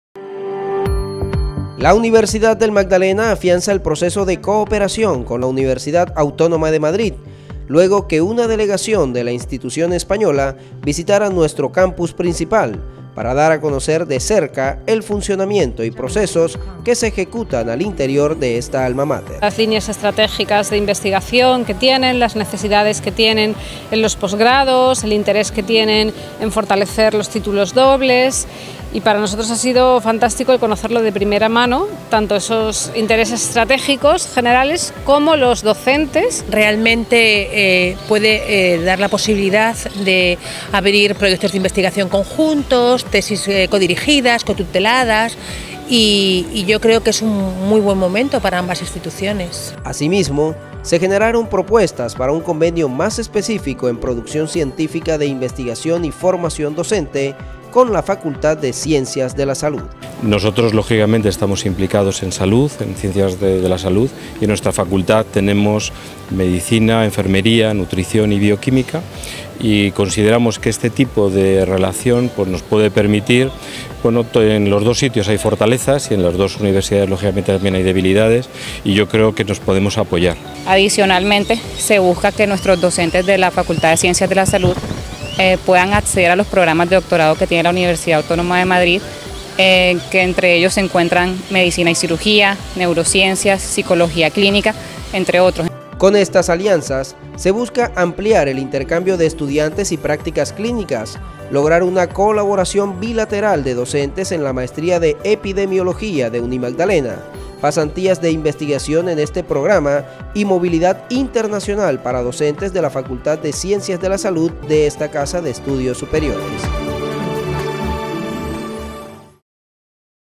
BA073_UNIMAGDALENA y la Universidad Autónoma de Madrid estrechan lazos de cooperación
Una importante delegación de la institución española visitó el campus principal de la Alma Mater y se reunió con el Doctor Pablo Vera Salazar, Rector de esta Casa de Estudios Superiores con el fin de avanzar en la actual alianza con la Facultad de Ciencias de la Salud.